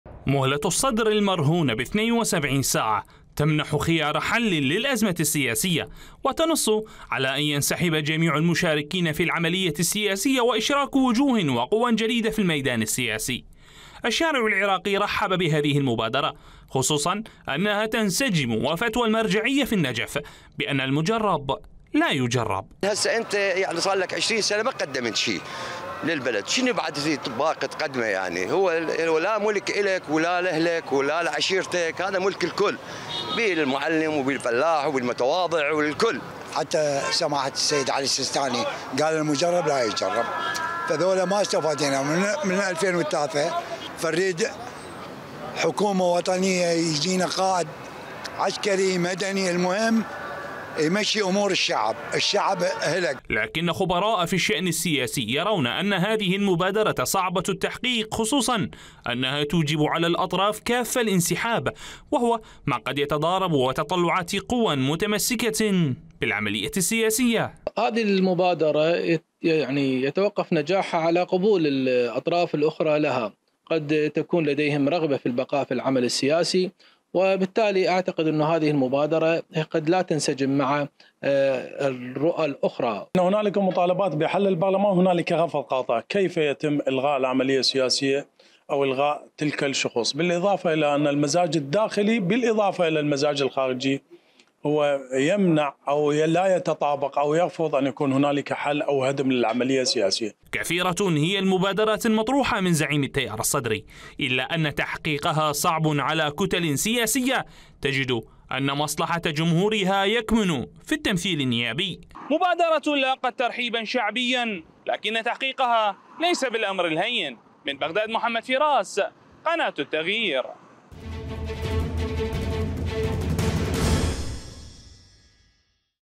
خطة مقتدى الصدر بعد مبادرة الـ 72 ساعة.. ماذا سيفعل ؟! | تقرير
ترحيب شعبي لمبادرة زعيم التيار الصدري التي تنص على تغيير جميع الوجوه والاحزاب المشاركة في العملية السياسية وادخال وجوه جديدة وسط ترجيح خبراء سياسيون بصعوبة تحقيقها.

موقع قناة التغيير الفضائية الالكتروني